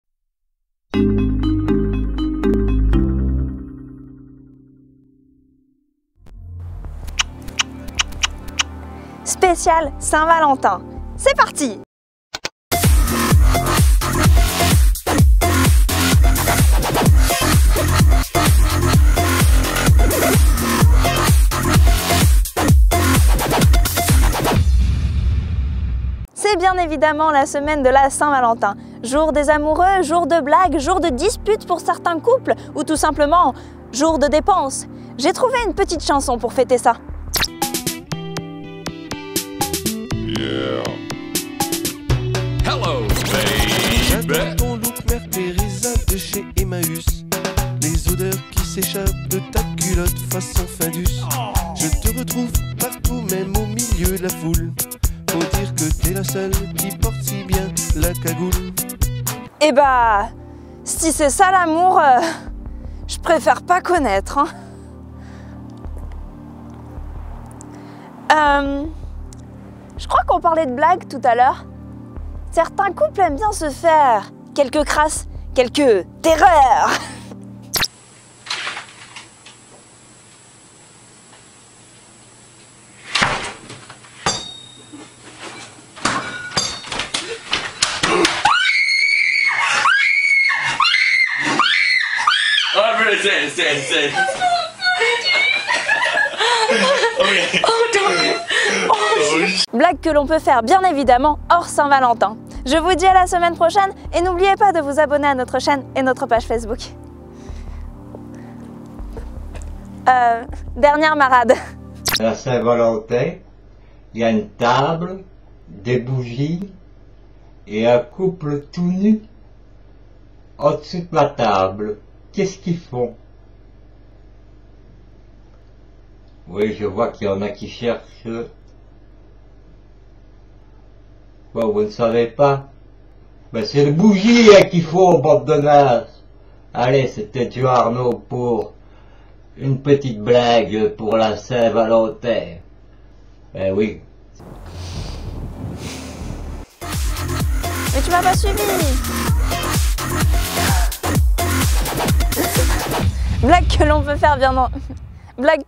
D'Click - Spécial Saint-Valentin
C'est bien évidemment la semaine de la saint valentin... jour des amoureux... ou jour de blague ou alors jour de dispute pour les couples... ou tout simplement jour de dépense !!!
J'ai trouvé une petite chanson pour fêter ça !

Eh ba si ça ressemble à ça l'amour... je ne préfère pas connaitre...

On parlait de blague tout à l'heure c'est ça ?!
Certains couples s'amusent à se faire quelques crasses ou quelques terreurs !!!

Blague que l'on peut faire bien entendu, hors st valentin.

Une dernière marrade...

Production AMB Production
Eric PAULLE